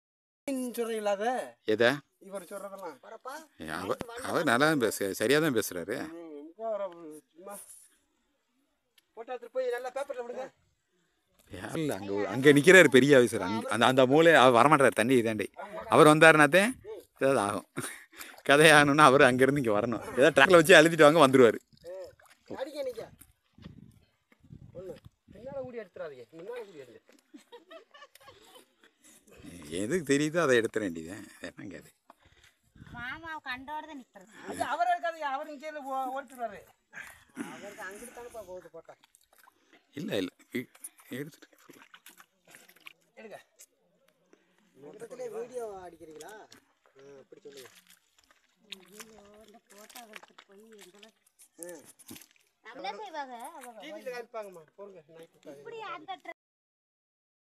Sustainable IPM Dry Chilli planting
Chilli seedlings raised from K1 seeds being transplanted in rainfed region near virudhunagar.